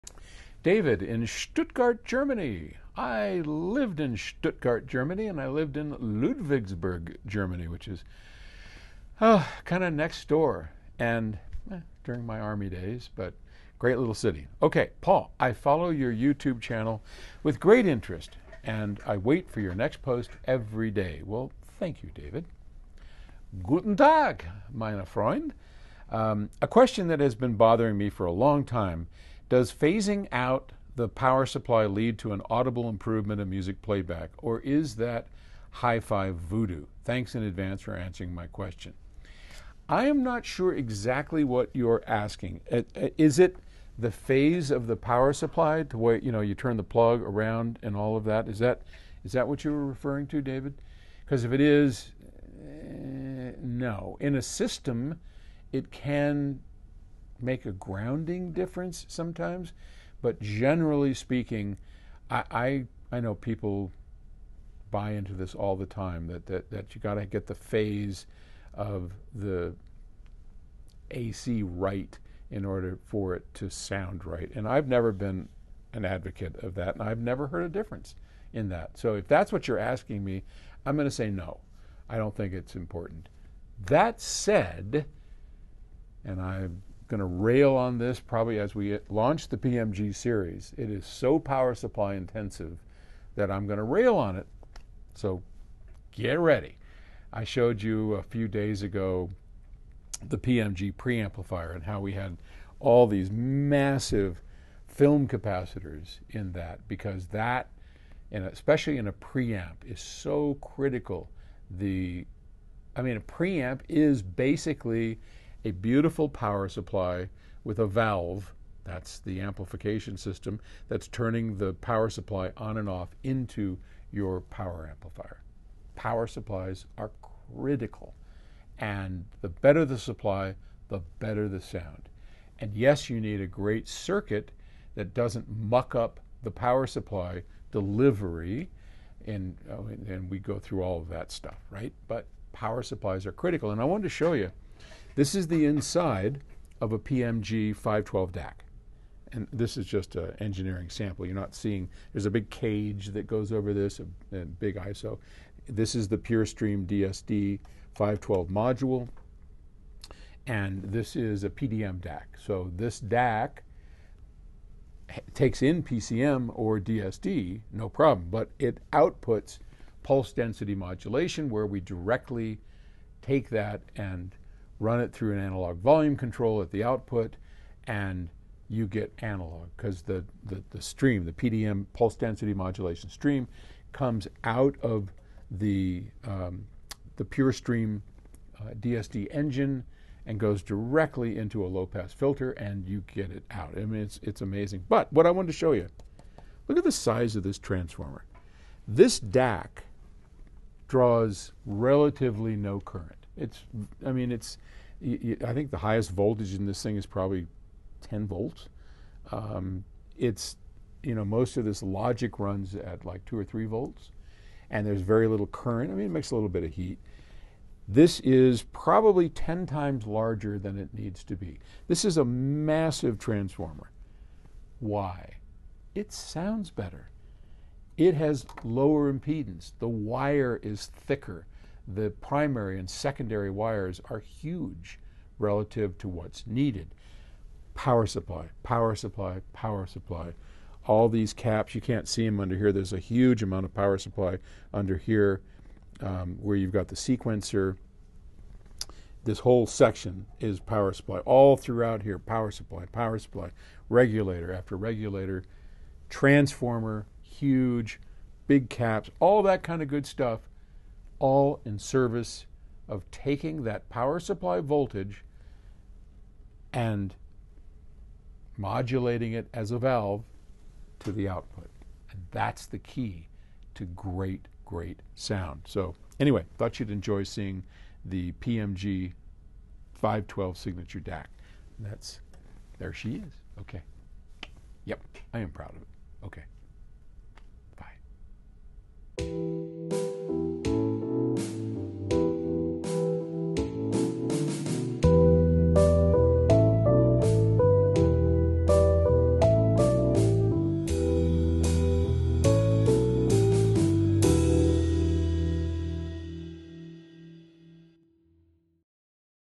The importance of power supplies and the PMG 512 insides
Have a look inside one of the world's most advanced DACs, the PMG Signature 512.